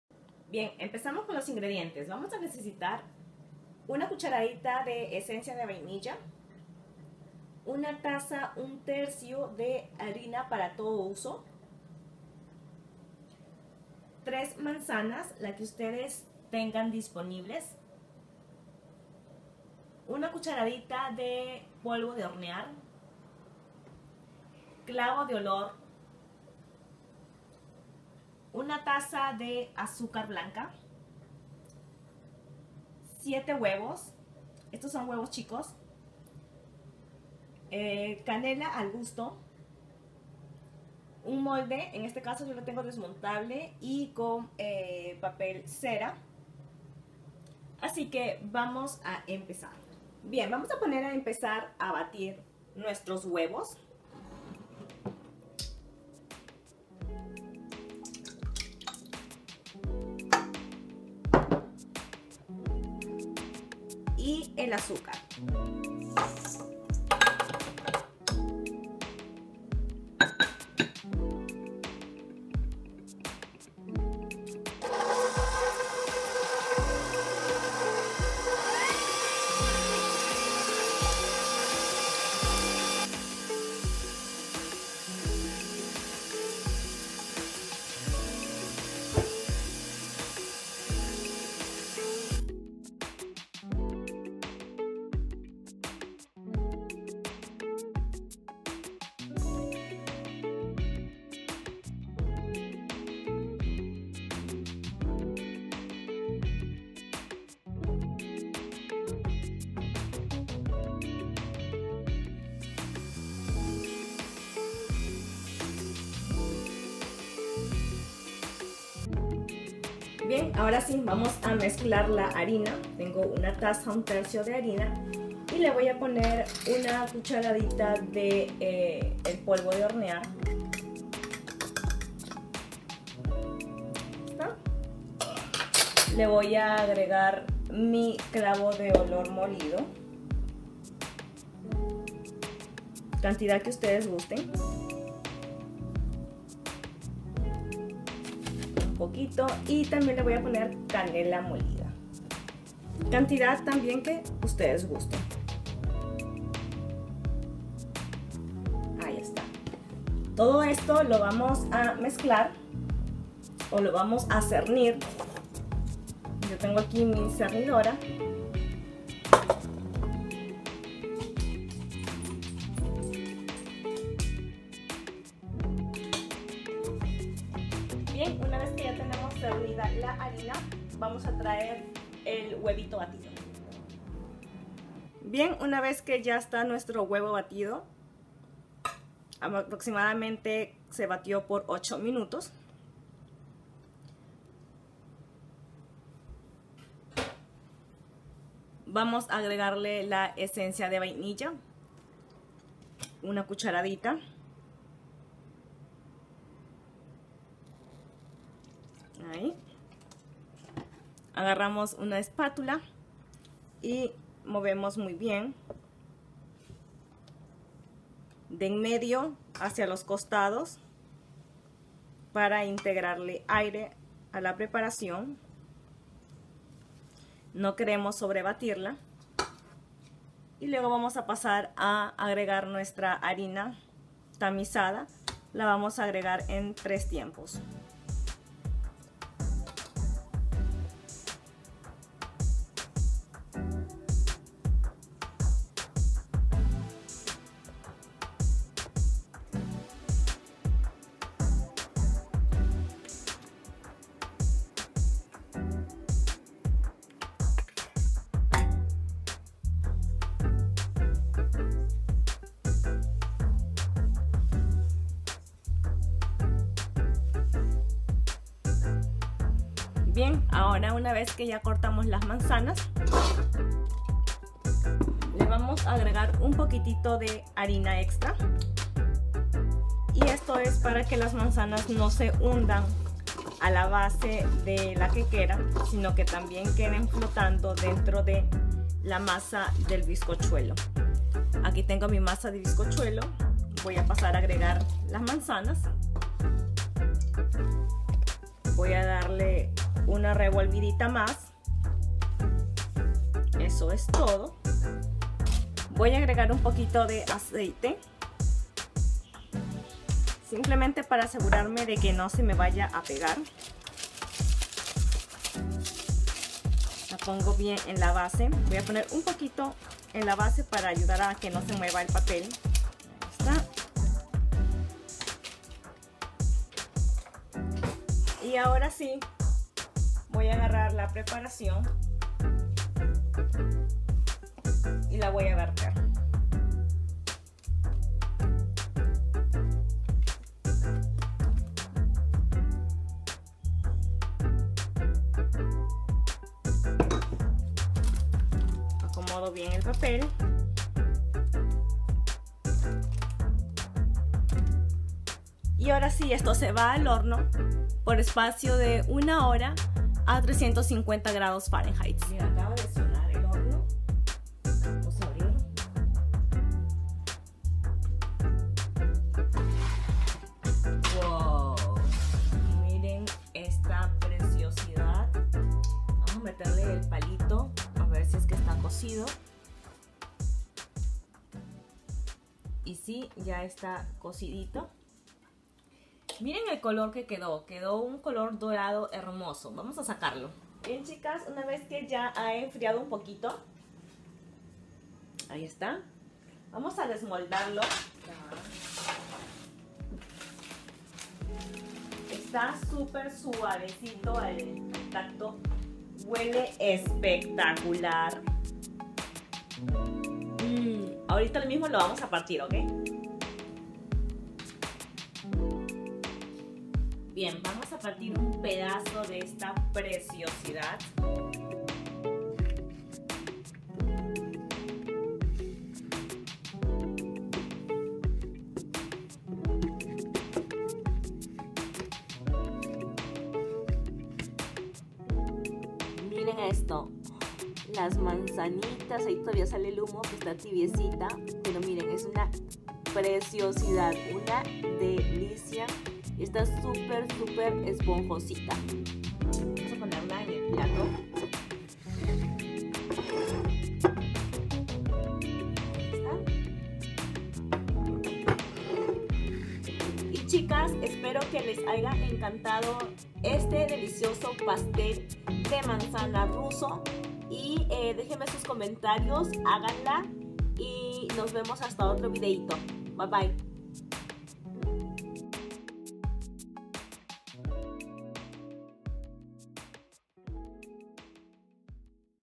Sharlotka Pastel de Manzana Ruso Super Facil
Hola chicas y chicos empieza la epoca navidena y les comprato una super rica receta de manzanas que les va a encantar!!!